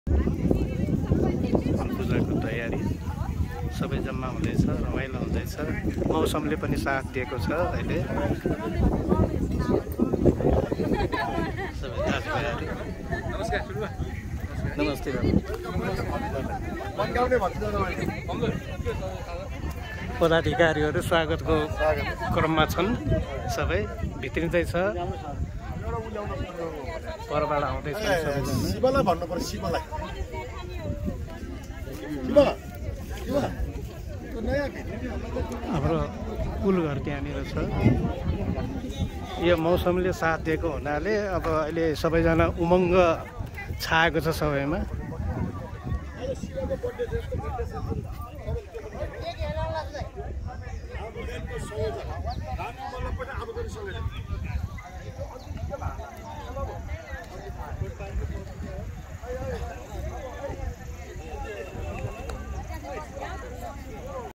Kulpuja BS 2080 Teenkilo
Kulpuja of BS 2080. Every year during the month of Baisakh or Jestha both the AGM of our organisation Ramyug Malla Pratishthan and Kulpuja are held. The organisation RYMF is our Santan's platform and a symbol of our Santan's unity. The clan head is Rajbir Malla from Patan Lalitpur.